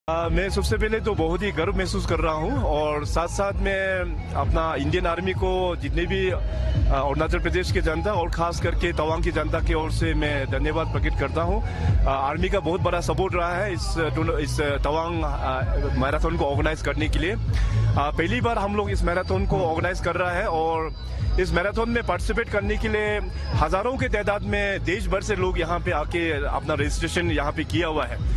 First high-altitude marathon a big success: Arunachal Pradesh CM Pema Khandu
First high-altitude marathon a big success, thanks to Indian Army’s support: Arunachal Pradesh CM Pema Khandu